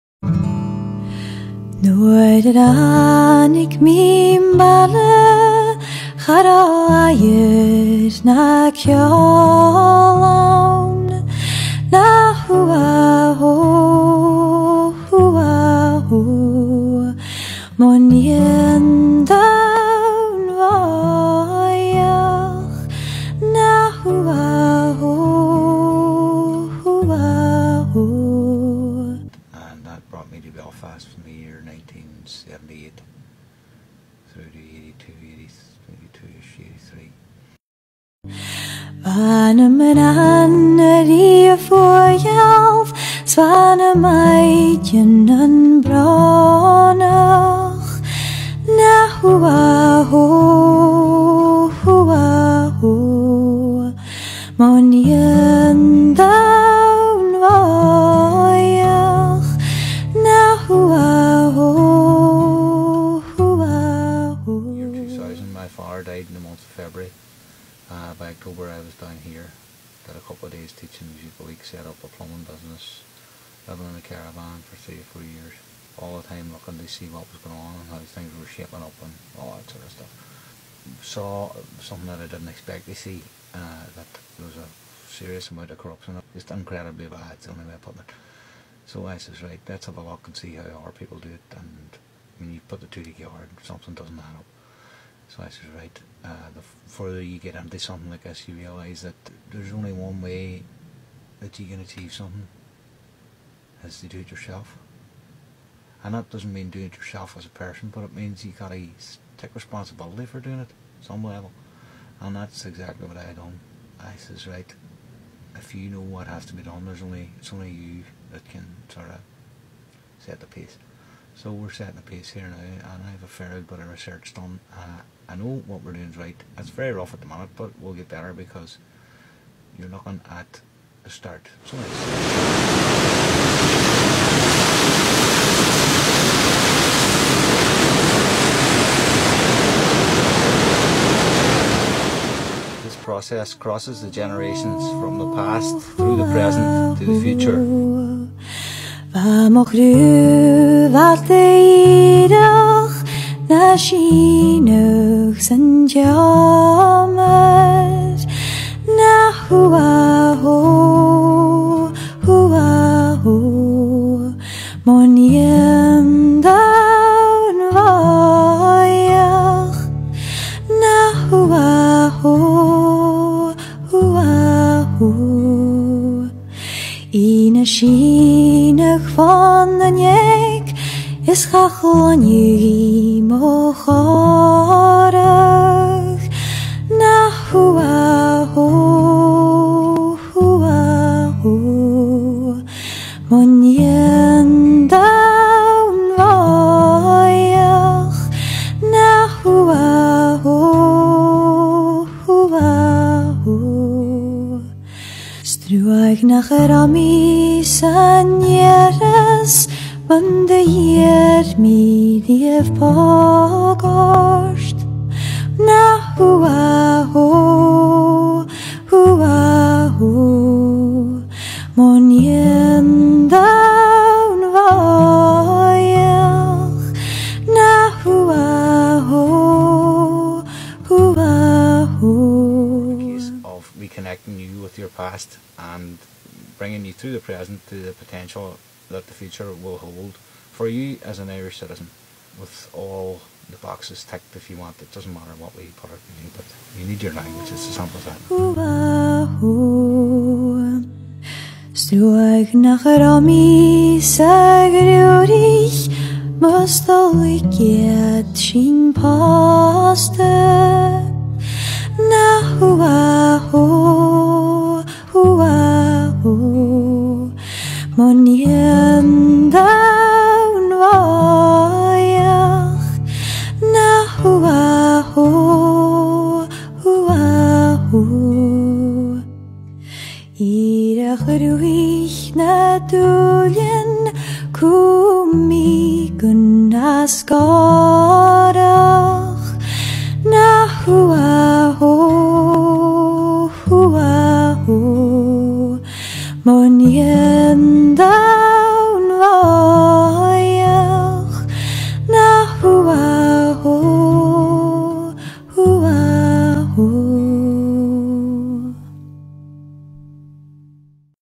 05-10Steps01
Here is a short intro to our work to create a just alternative to Ireland's rotten state.